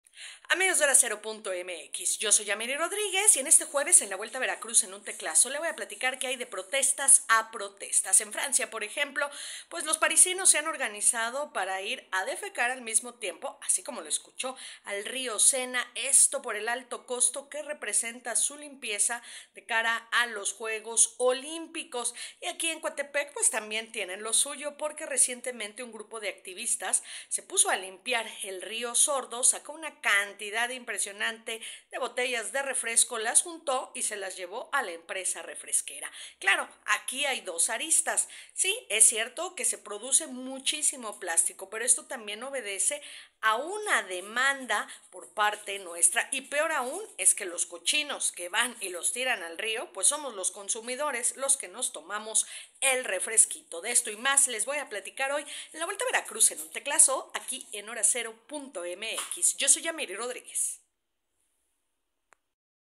Del Sena al Sordo, las protestas radicales / Yamiri Rodríguez Madrid
Esta es la columna La vuelta a Veraruz en un teclazo, de Yamiri Rodríguez Madrid; hoy, Del Sena al Sordo, las protestas radicales